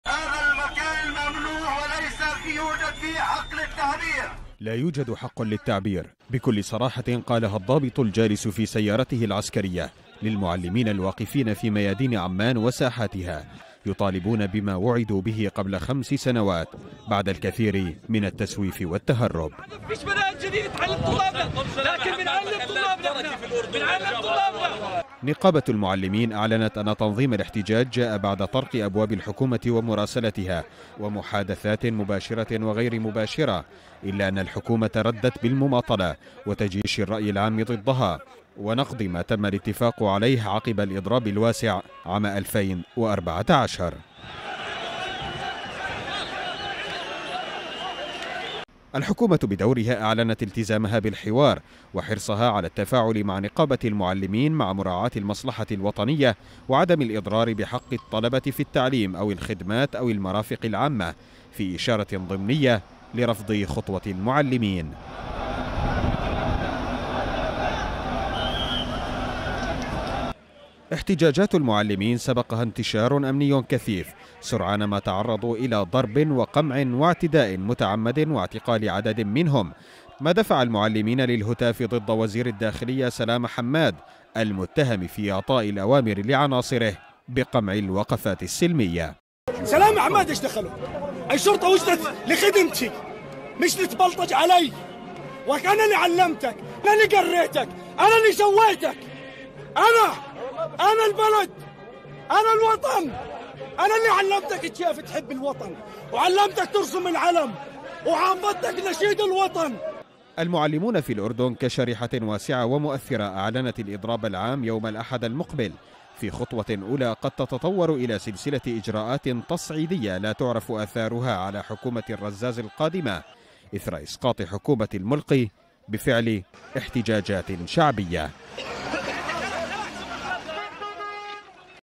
معلمو الأردن في مواجهة مع حكومة الرزاز
حكومةُ الرزاز التي جاءت بعد احتجاجاتٍ شعبيةٍ أسقطت الحكومةَ السابقة في مواجهةٍ مفتوحةٍ مرةً جديدةً في الشارع .. هذه المرةُ مع المعلمين الذين يطالبون بعلاوةٍ مستحقةٍ بعد الاتفاقِ عليها مع حكومةٍ سابقة، إذ يخوضُ هؤلاء إضراباً يومَ الأحد المقبل في خطوةٍ تصعيديةٍ بعد الاعتداءِ عليهم من قبلِ قواتِ الأمنِ أمسِ في ميادينِ وساحاتِ عمّان التي شهدت اختناقاً مرورياً بسبب الانتشارِ الأمني المكثف.